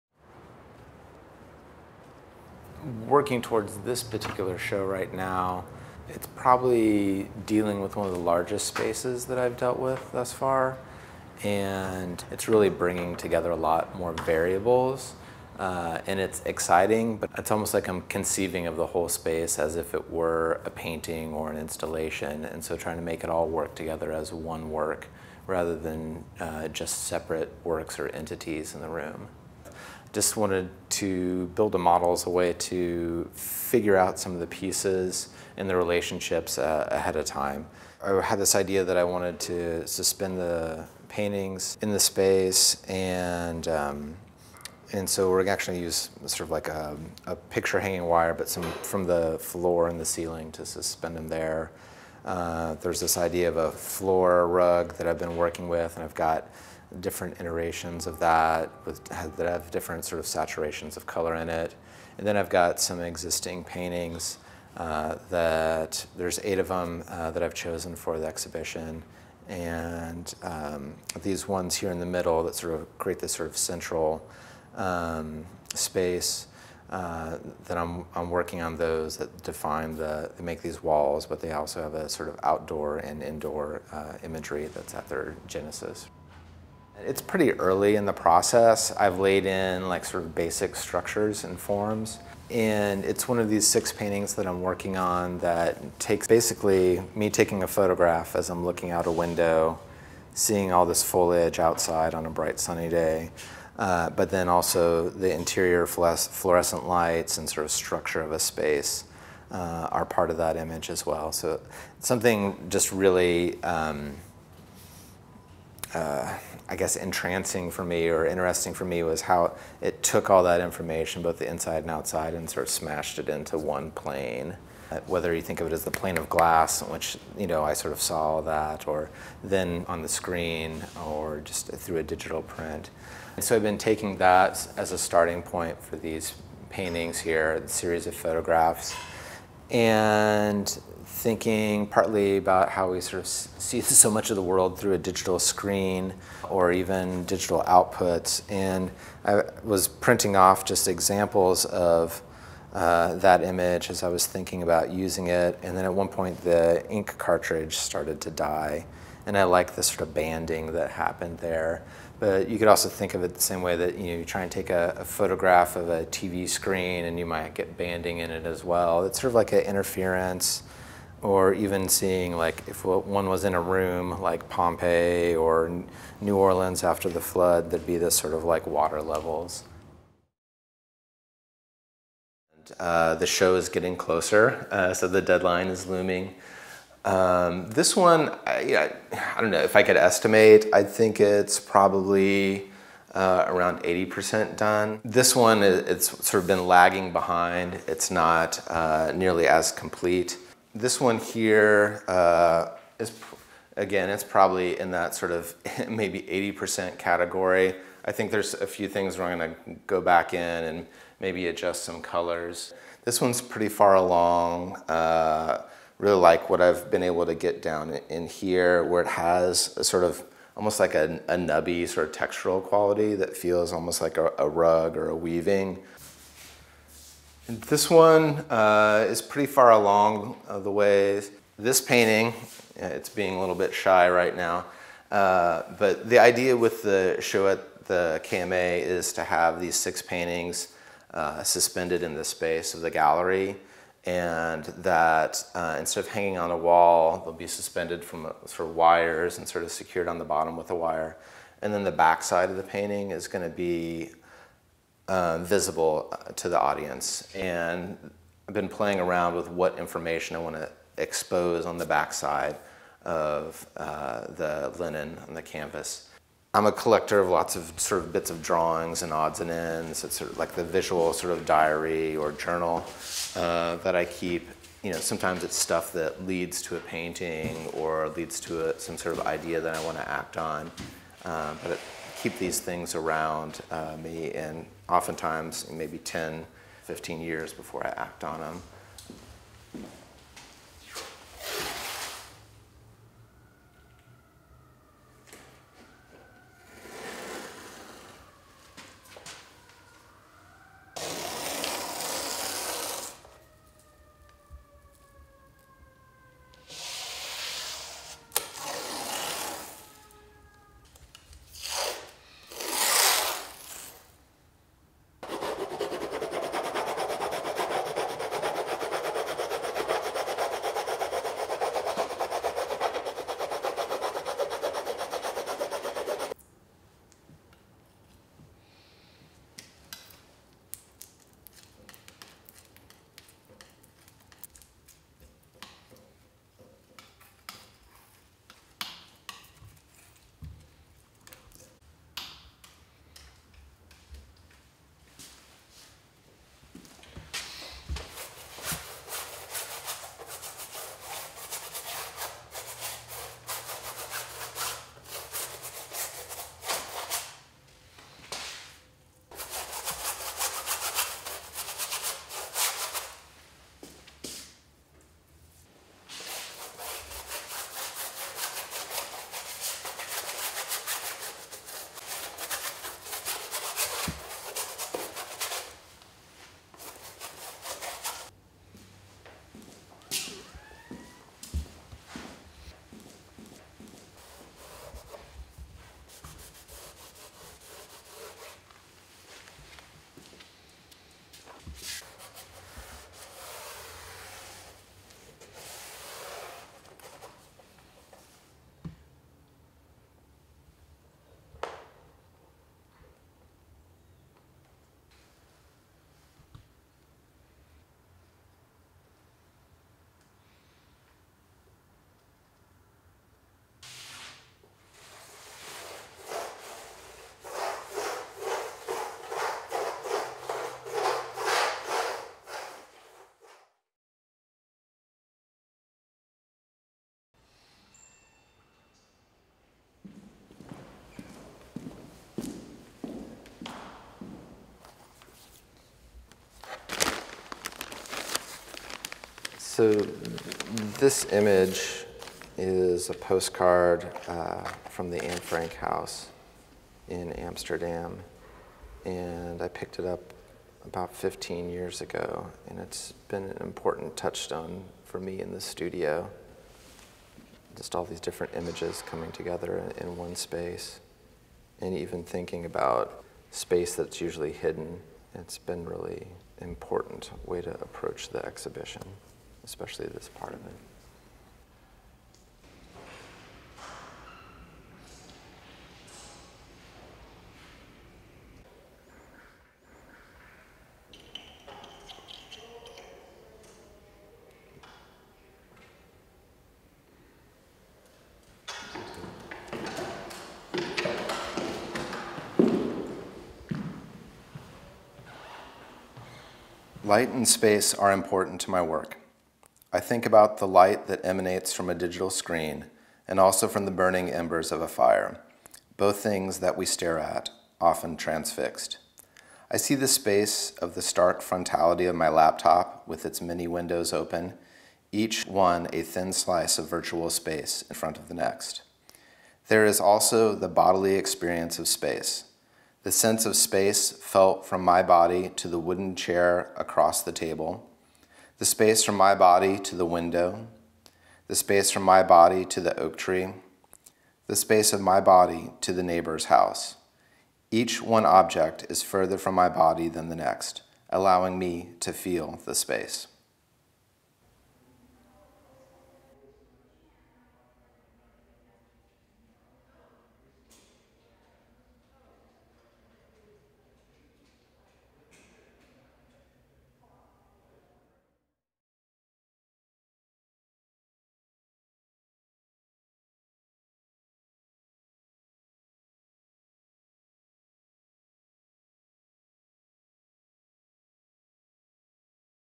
Jered Sprecher: Outside In
A short artist video documenting painter Jered Sprecher's preparations for his first solo museum show, at the Knoxville Museum Art. Filmed and edited by Paul Harrill.